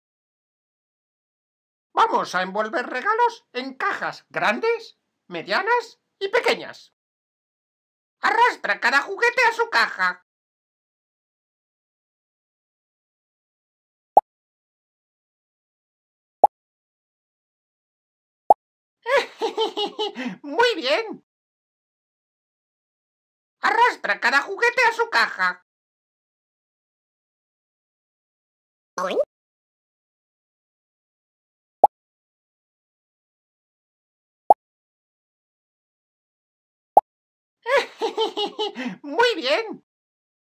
ABN - Regalos grandes, medianos y pequeños
En este juego se trabaja el tamaño teniendo en cuenta la apariencia de los juguetes y de las cajas (grandes, medianos y pequeños).